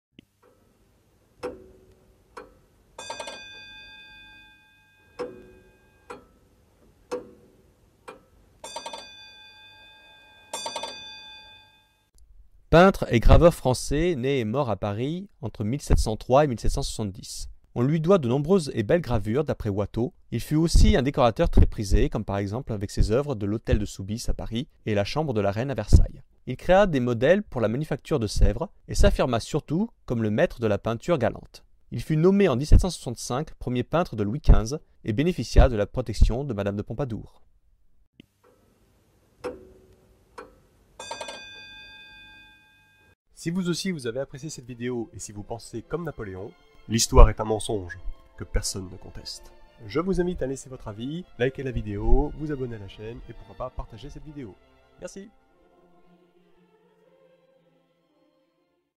[QUI EST FRANÇOIS BOUCHER ?] LA MINUTE NECESSAIRE DE L'HISTORIOGRAPHE.
Aujourd'hui, François Boucher, (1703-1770), peintre français.